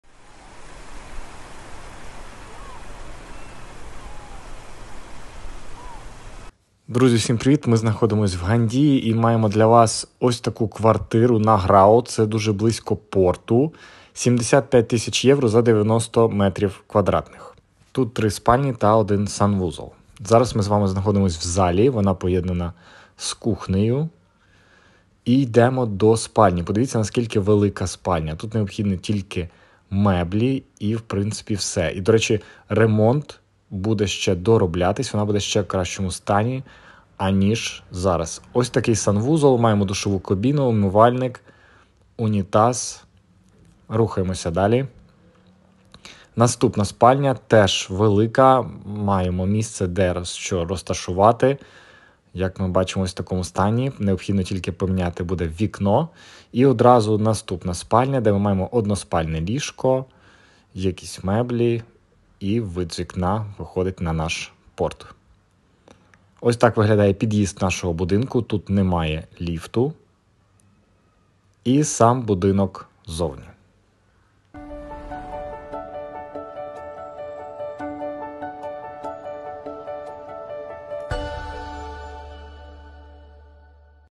Gandia, Valencia, Alicante 🔥75.000🔥€ 90м2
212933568

 ✅🔥75.000🔥€ вартість квартири;
✅ Орієнтовна вартість здачі в оренду після купівлі, становить 750 € на місяць!

✅ 3 спальні + вітальня + кухня
📏 Площа - 90 м2
🚽 Санузлів - 1
🏢 Поверх - 5/5 (укр.)
🛗 Без Ліфту

Переоформлення:

- 10% податок на нерухомість;
- ріелторська комісія;
- нотаріальне переоформлення;
- страхування нерухомості;

Точну суму переоформлення «під ключ» вам вирахує персональний менеджер!
Звертайтеся за безкоштовним прорахунком та консультацією по цьому об’єкту👇👇👇
🏠Адреса: Gandia,
Carrer Eivissa, 73

🏖 Знаходиться неподалік від пляжу. До моря - 1,2 км.

✅ Gandia - це місто недалеко від Valencia, яке розташоване поряд з найкращими 🏖пляжами в Іспанії!
🛬Ваш запасний аеродром на будь-який час